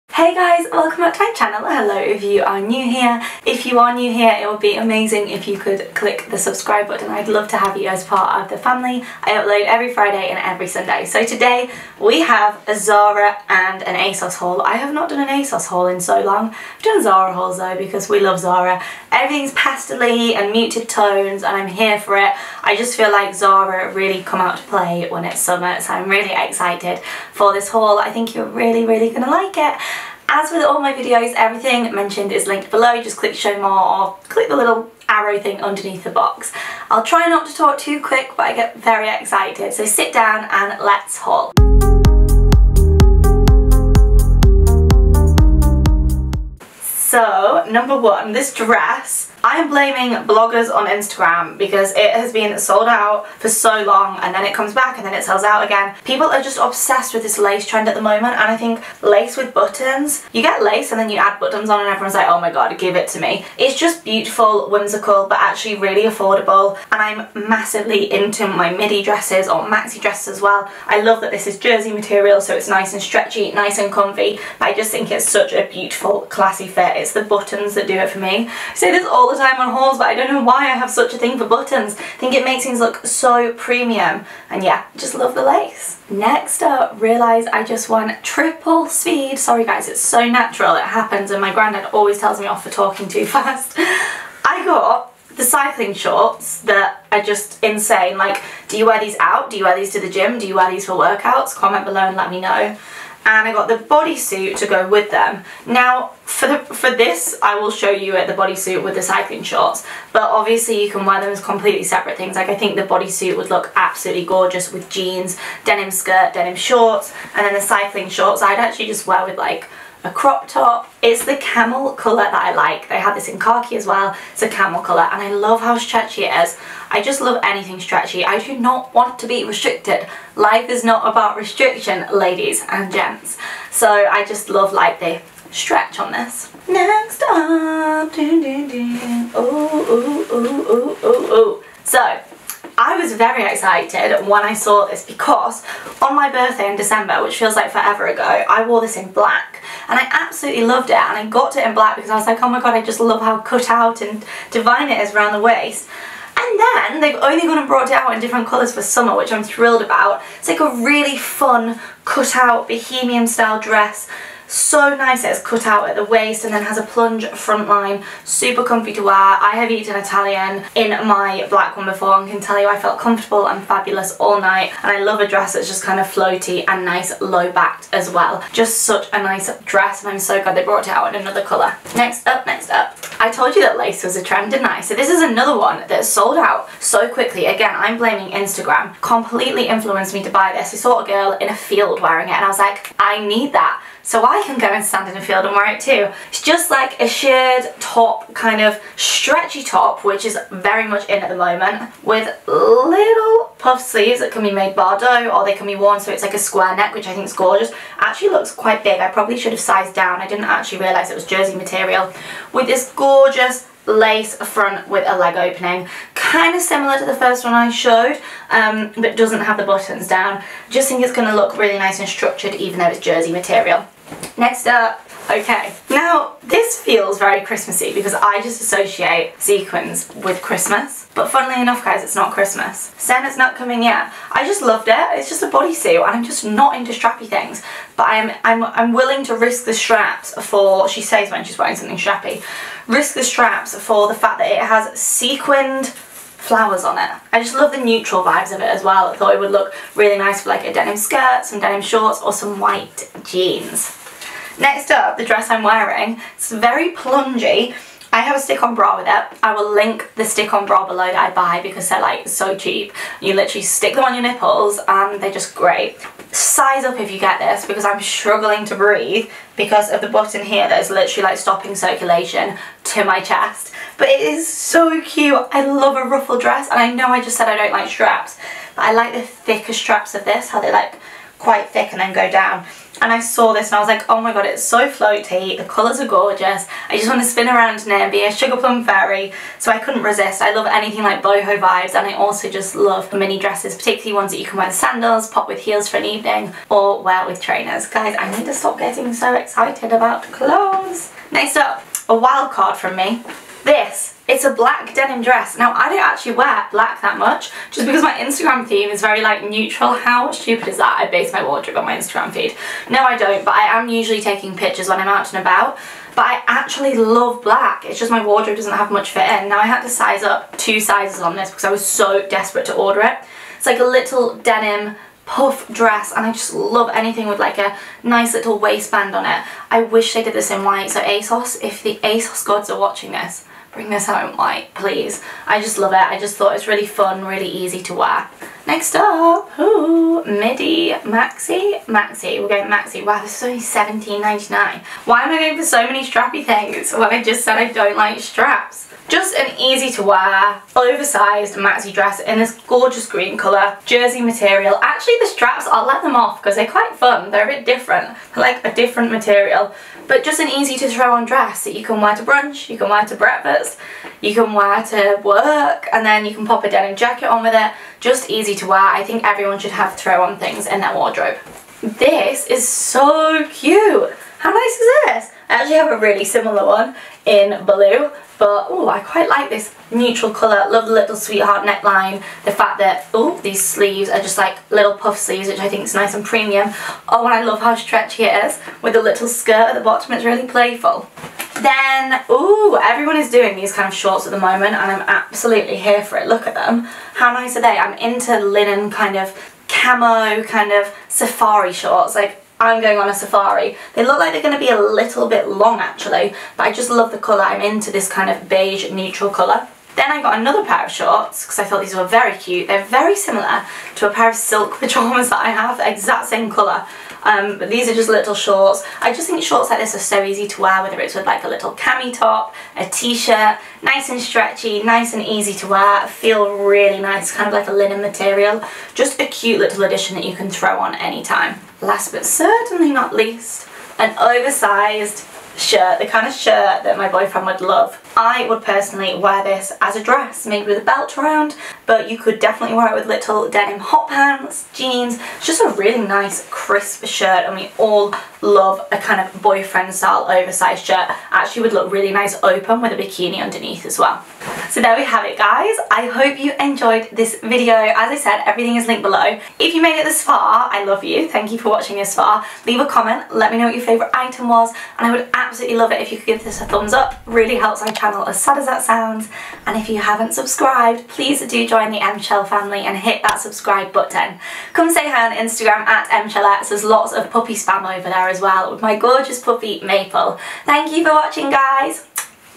ZARA & ASOS Try On Haul 2020 | Em Sheldon
Hello it’s time for a Zara and Asos try on haul let’s see what’s new, you can find links to everything I unboxed below. ALL items linked under this video. Instagram: @EMSHELX

- Ad own product

_______________ ITEMS MENTIONED _______________

Stick on bra

Similar bodysuit

FAQS

——— MY EVERY DAY BEAUTY FAVES ———